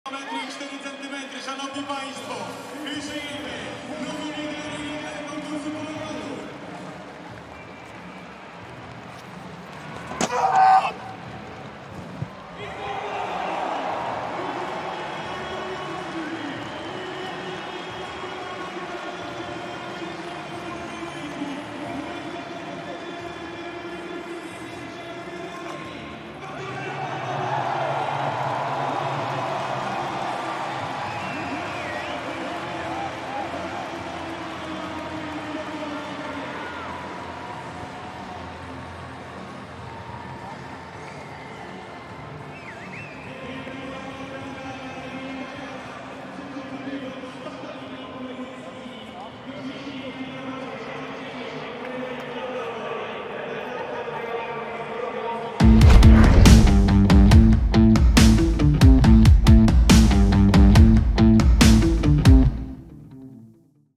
DWUKROTNIE POPRAWIŁ REKORD ŻYCIOWY i wygrał w Chorzowie!
Konrad Bukowiecki wygrywa konkurs, pchnięcia kulą, podczas 10. jubileuszowej edycji Memoriału Kamili Skolimowskiej (2019 r.). W pierwszej kolejce uzyskał wynik 22.04, bijąc własny rekord Europy do lat 23, natomiast w drugiej wyśrubował go, posyłając kulę na 22.25!

✅ Upewnij się, że zostawiłeś follow po więcej takich materiałów!